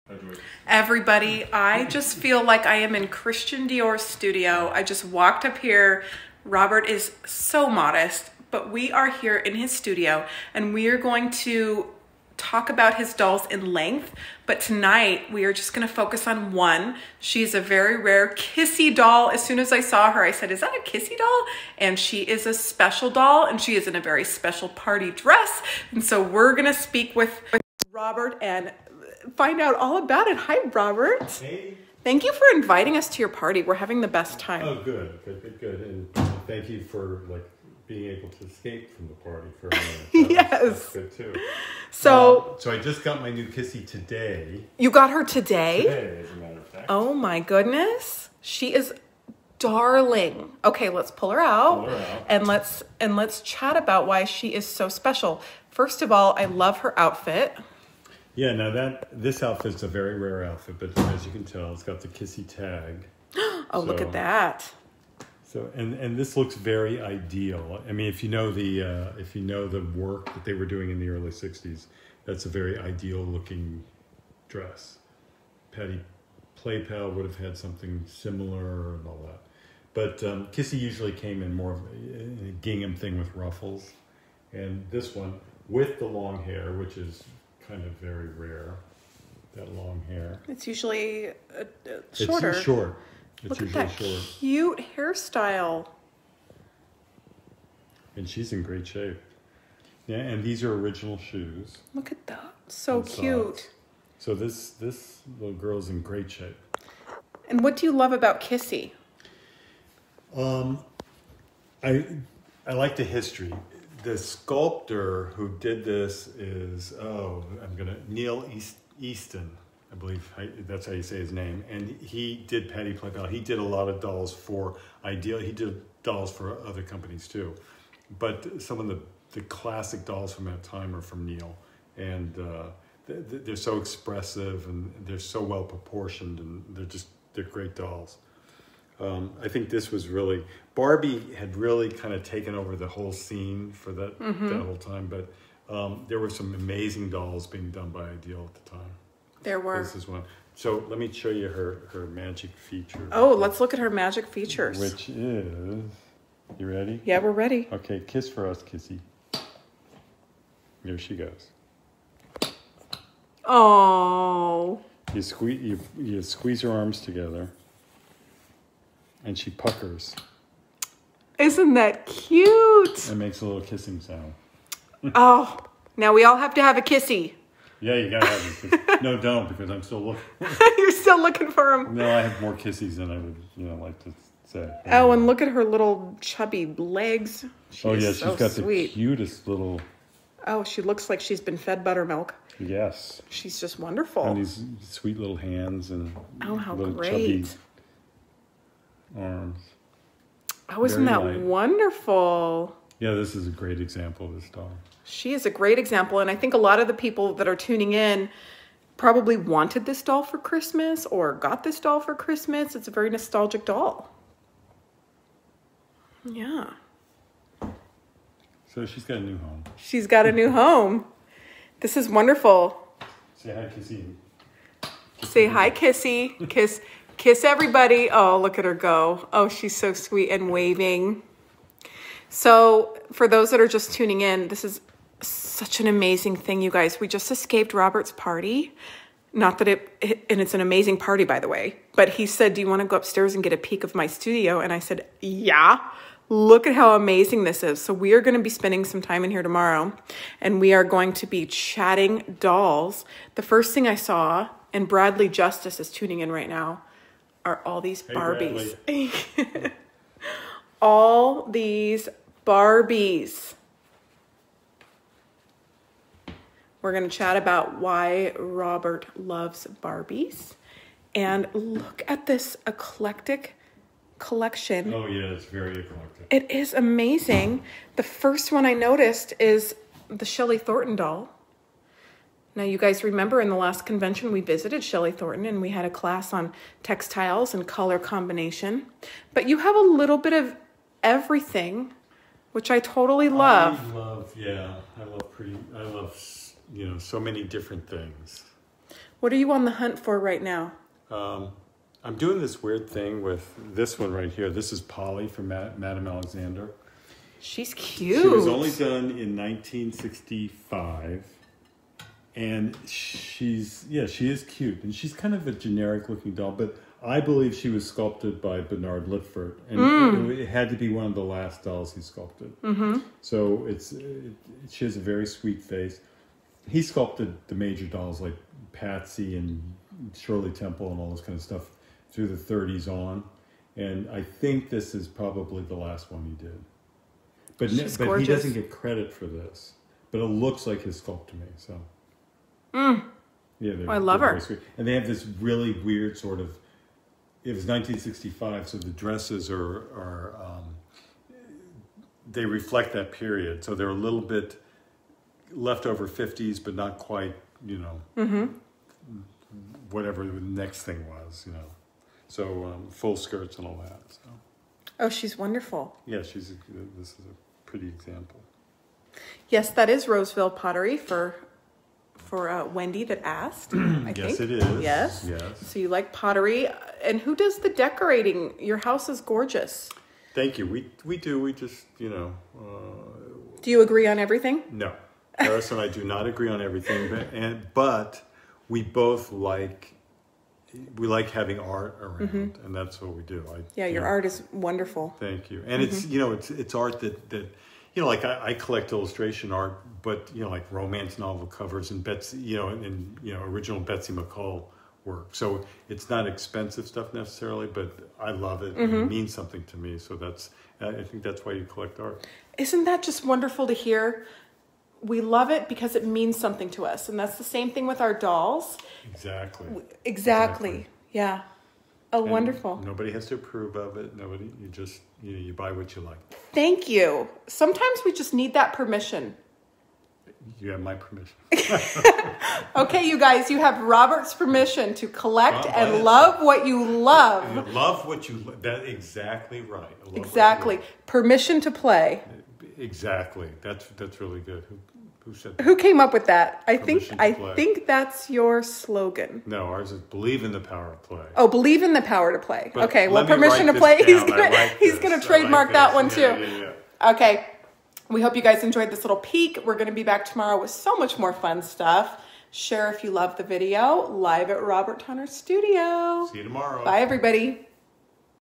Robert Tonner and a Rare Kissy Vintage Doll | Vintage Doll Video
Robert Tonner collects all kinds of dolls but he especially loves Kissy dolls. In this video, he shares one of his new favorite additions with Rachel Hoffman. We were in town filming content for the January 2020 Virtual Doll Convention. This was so much fun!

Info from Just Collecting.com:

The Kissy range of dolls included a number of different designs and sizes. These were:

    Kissy – Produced from 1961 until 1964, the original Kissy doll is a toddler measuring around 22” tall. When the doll’s arms are squeezed together it makes a kissing sound and its lips pucker up.

    Tiny Kissy – Produced from 1963 until 1968, Tiny Kissy is a smaller 16” version of the original Kissy doll. It features the same kissing feature as the larger version and wears a similar red and white gingham dress. In 1966 the company redesigned the doll, with long straight auburn hair in a side parting and orange and yellow A-Line dress.

    Baby Kissy – Produced from 1963 until 1964, the doll is a younger, ‘baby’ version of the original Kissy doll with bowed legs so that it sits rather than stands.

    Cuddly Kissy – The Cuddly Kissy doll was a smaller, cloth-bodied version of the Baby Kissy doll, produced for one year in 1965. The kissing feature was activated by squeezing the doll’s tummy, which would draw the hands together and activate the kissing noise.